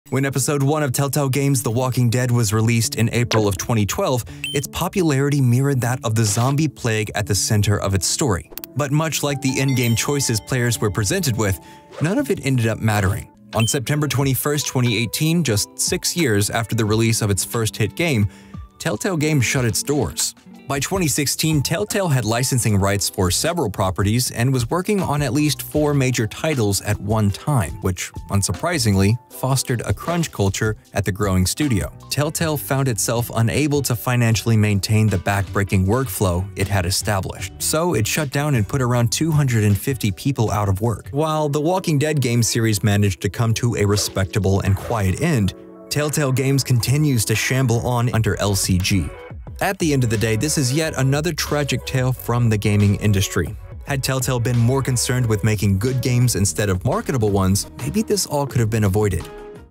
the cursed half life of telltale's the walking dead
Telltale Games became so popular after The Walking Dead, they started to create games for other beloved properties. But the flame would soon start to go out.  While Telltale had a fast and steep climb to success, it had an equally hard fall from grace.